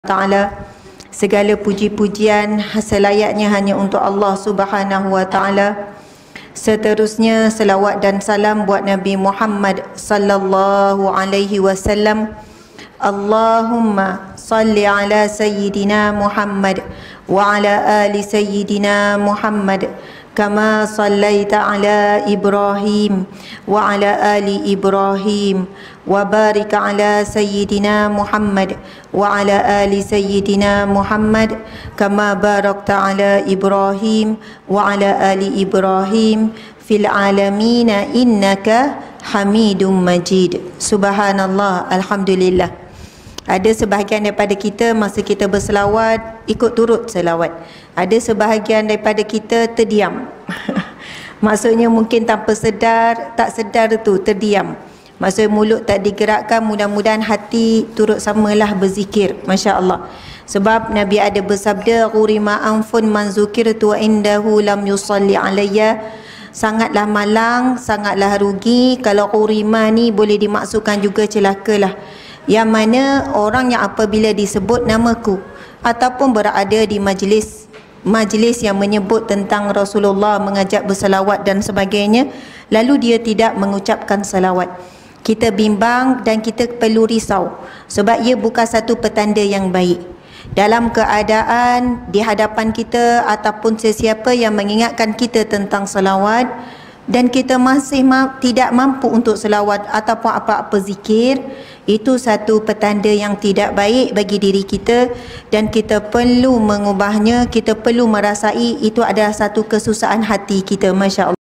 Tidak berselawat ketika nama Baginda Rasulullah S.A.W disebut - Ustazah Asma' Harun
Ustazah Asma' Harun
Petikan dari Kuliah Asar Perdana
Tajuk : Tanda Lemah Iman